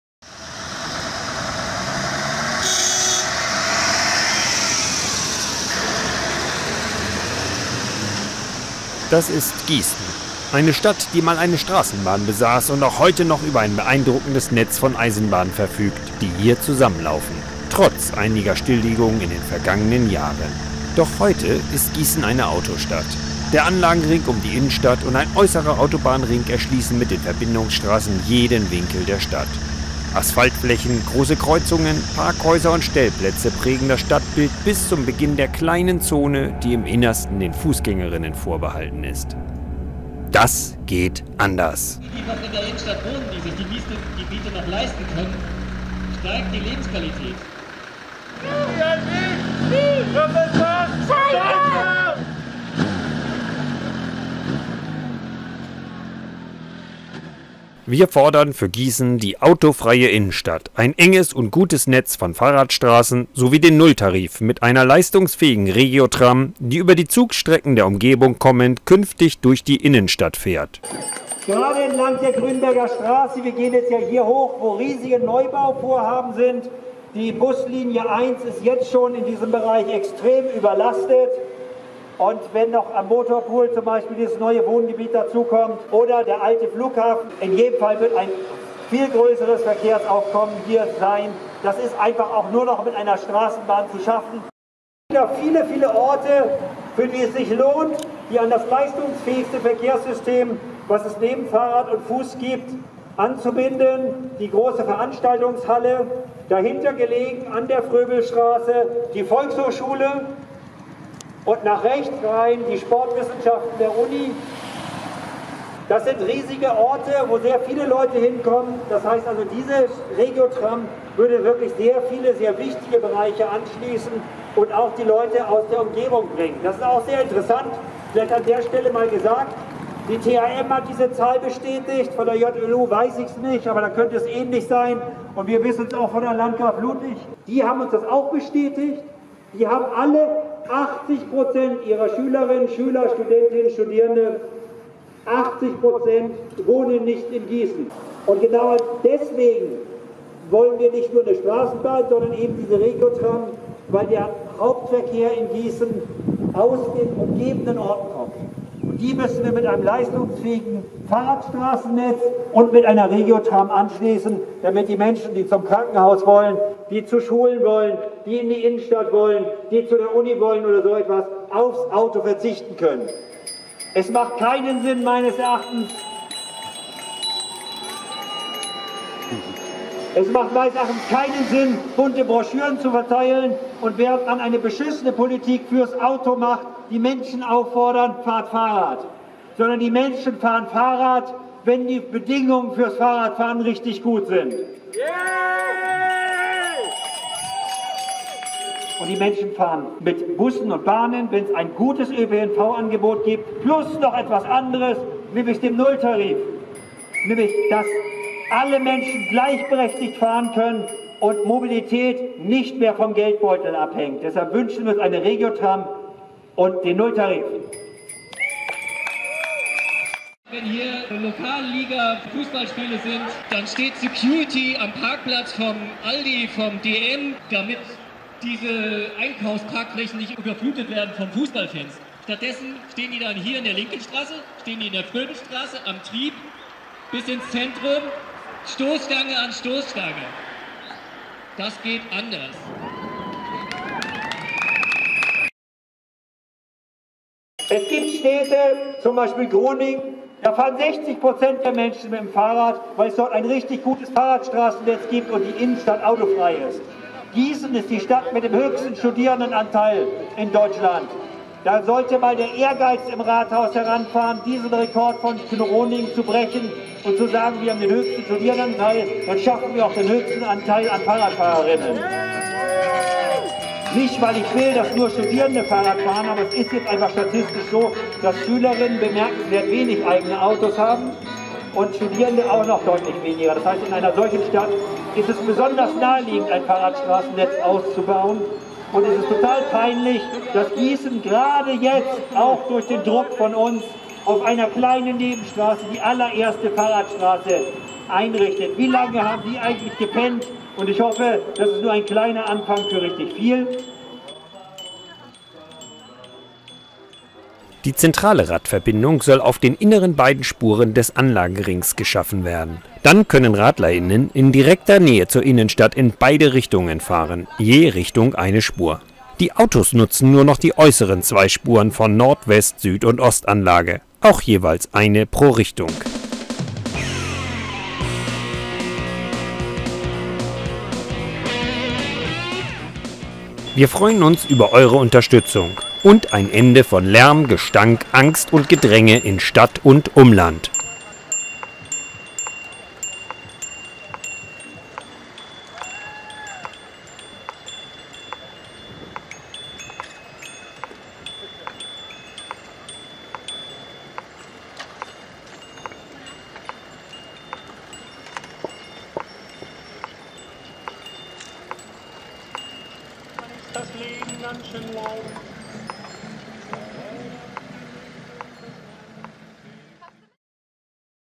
Für Fahrradstraßen und RegioTram: Verkehrsaktionstag am 25.4.2020 in Gießen
Der eigentlich geplante, ganz große Aktionstag ist es nicht geworden. Corona machte einen mächtigen Strich durch die Rechnung. Ob überhaupt was gehen würde, testeten die Verkehrswende-Aktiven mit einer Demoanmeldung Anfang April. Die wurde verboten, doch mit einem geschickten Manöver um einen viertägigen Protest für Grund- und Menschenrechte setzen sie per Bundesverfassungsgerichtsbeschluss das Demorecht wieder durch. Dem folgte nun der Verkehrsaktionstag am Samstag, den 25.4. - mit einer Fahrrademo um 12 Uhr auf dem als Fahrradstraße geforderten inneren Anlagenring und einer bunten "Latsch"demo für den Bau einer Straßenbahn auf der Grünberger Straße ab 14 Uhr. Selbstverständlich wurden alle Abstands- und Hygieneregeln eingehalten. Das durch das Verfassungsgerichtsurteil ausgebremste Ordnungsamt befand dann auch: Alles in Ordnung. Und das Thema Verkehrswende ist endlich wieder auf der Tagesordnung, die wochenlang nur von Corona, Wirtschaftsrettung und lautem Nachdenken über Rücknahme von Klimaschutzzielen geprägt war.